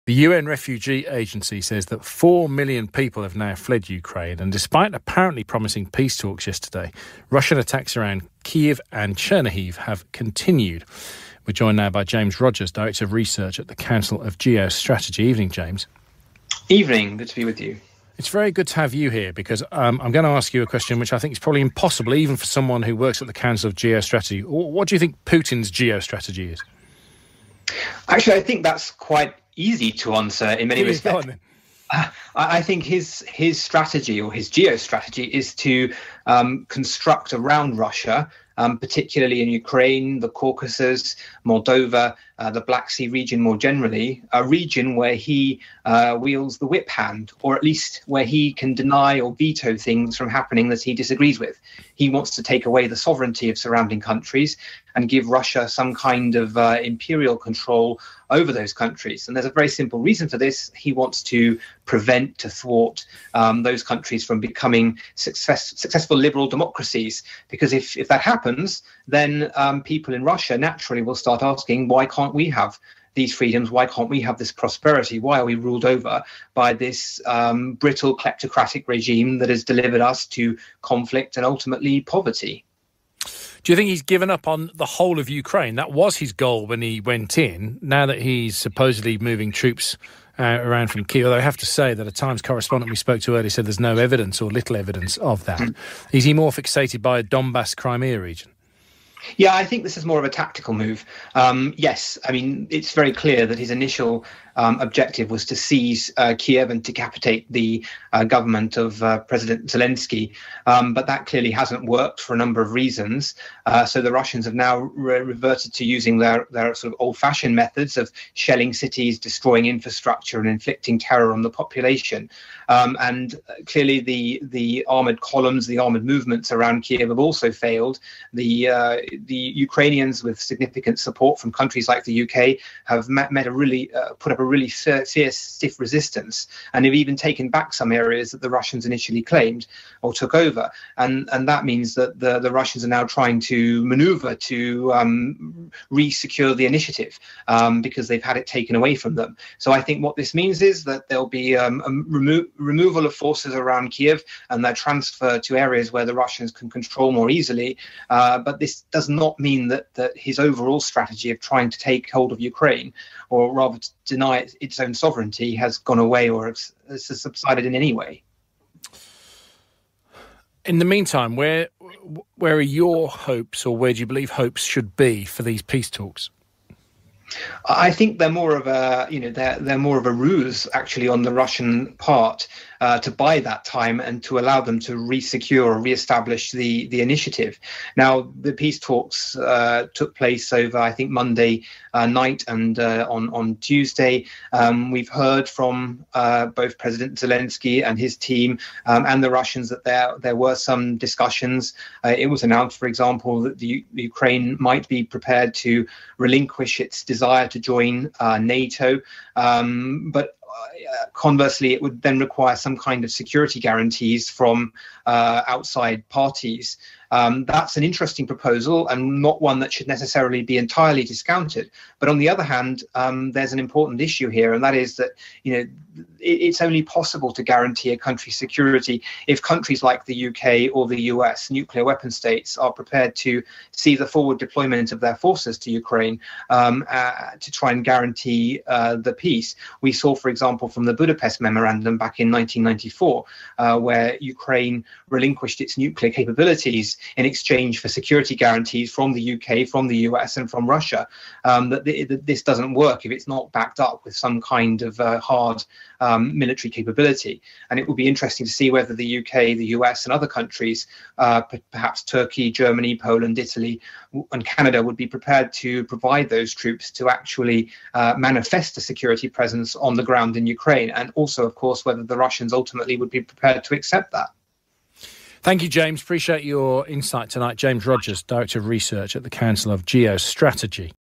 James Rogers speaks to Times Radio about peace talks, Russian military strategy and more
On 30th March, our Director of Strategy spoke to Phil Williams of Times Radio about what Putin's geostrategic goals are with the war in Ukraine, whether or not he's achieving them and how real the prospect of peace talks is.